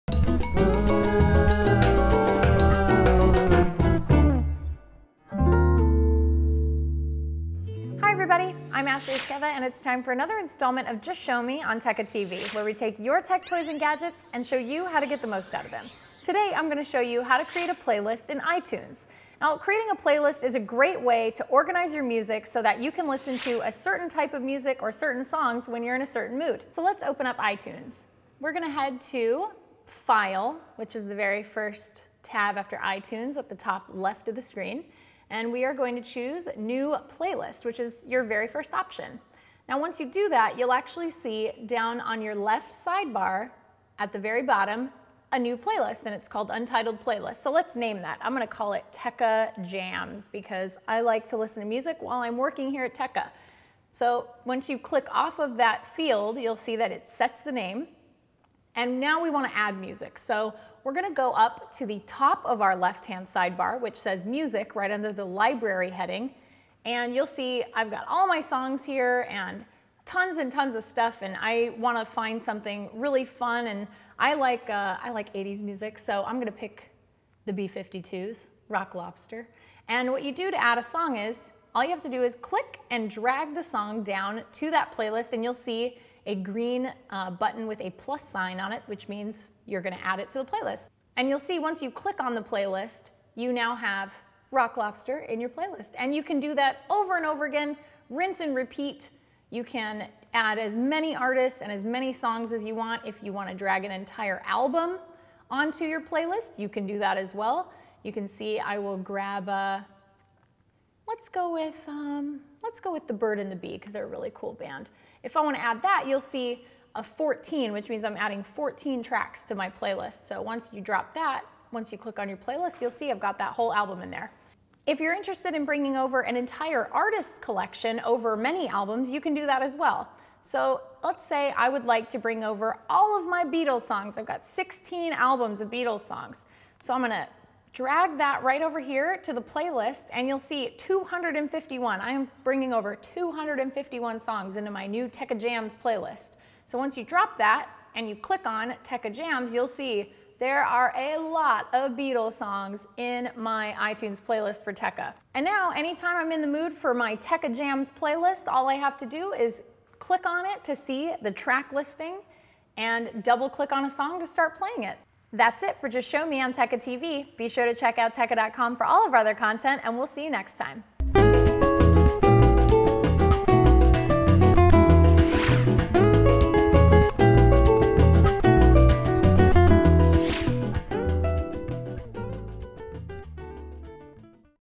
Just Show Me: How to create a playlist in iTunes
In today's episode we'll show you how to create a playlist in iTunes.

Playlists are a great way to organize your favorite music so that you're only listening to the tracks you want to. With iTunes it's easy to set these lists up. It's only a mater of dragging and dropping the songs or albums you want into your playlists; we'll show you step by step in our video.